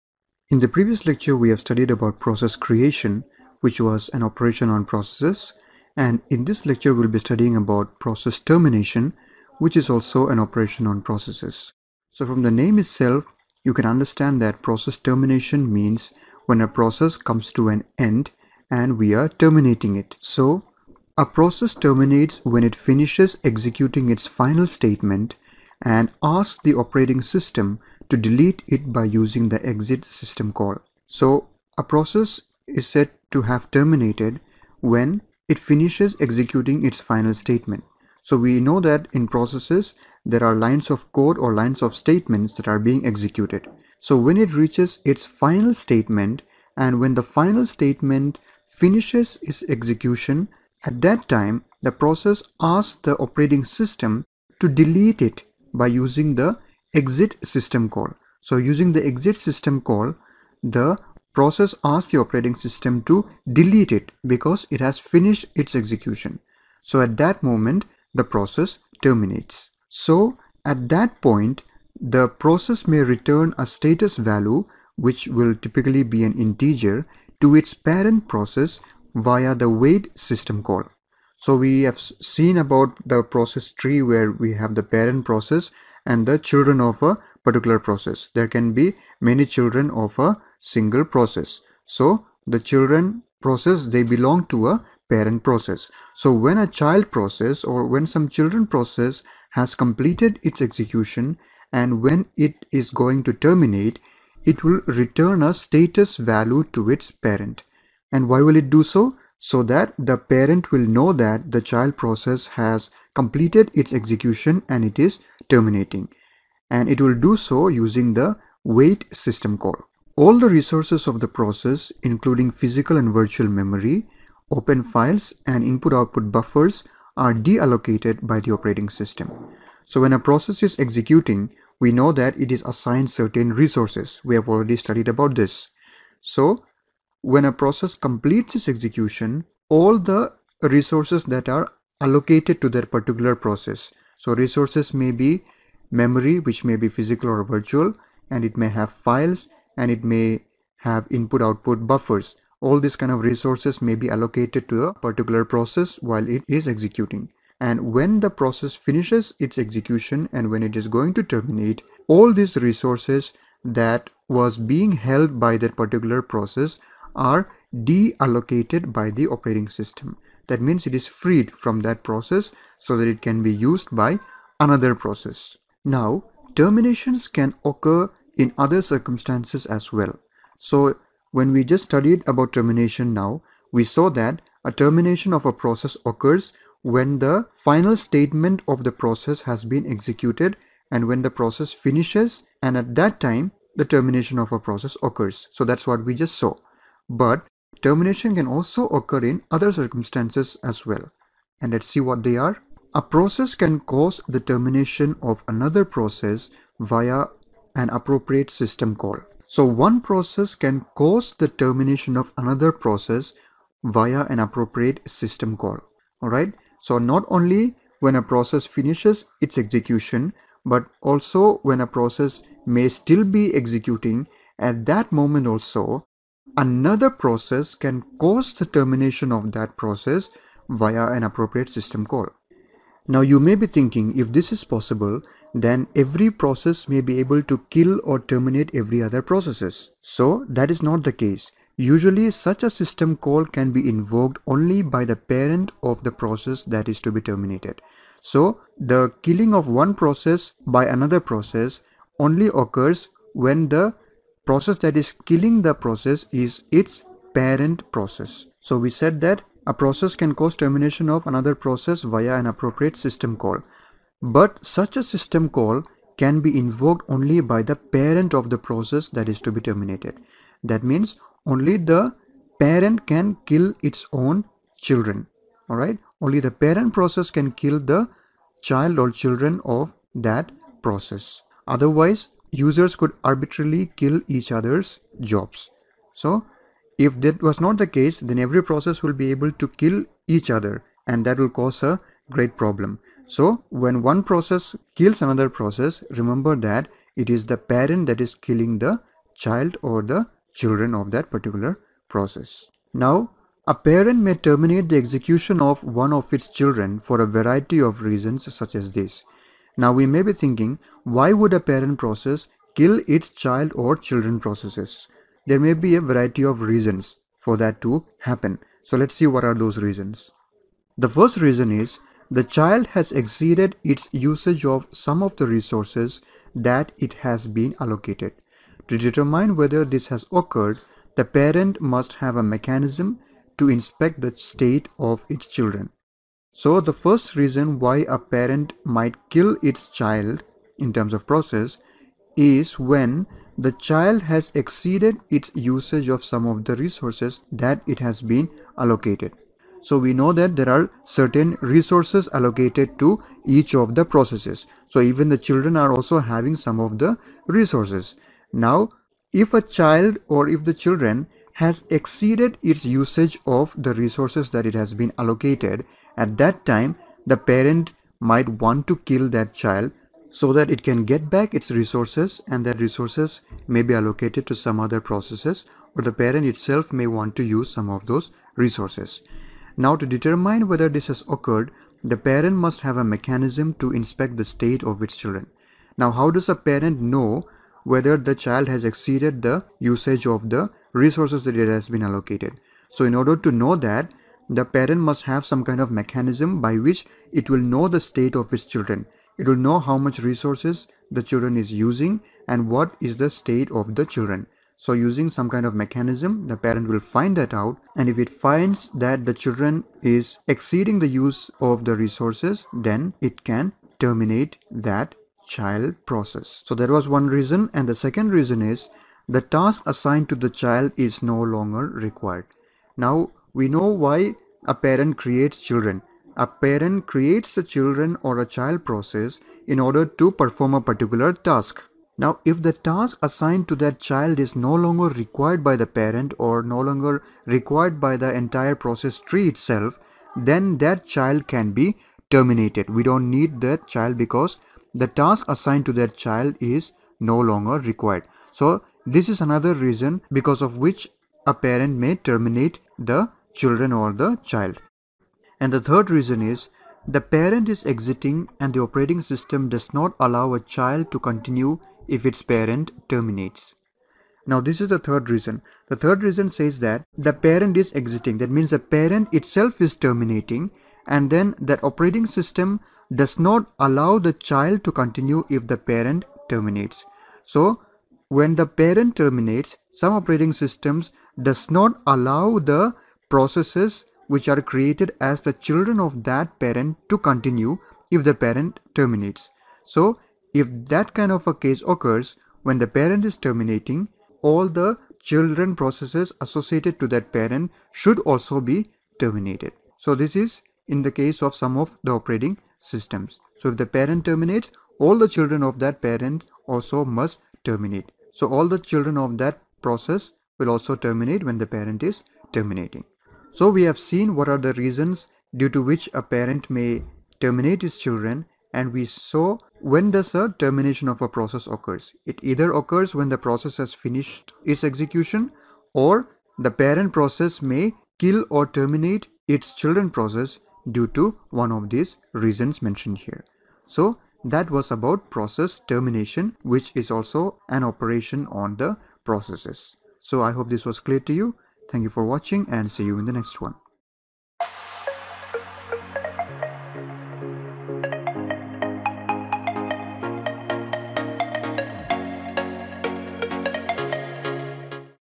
Operation on Processes – Process Termination
Operating System: Operation on Processes – Process Termination
Topics discussed:
OPERATION ON PROCESSES:
Process Termination-
• Termination of process.
• Reasons why termination occurs.
• Reasons why a parent process may terminate its children processes.

Follow Neso Academy on Instagram: @nesoacademy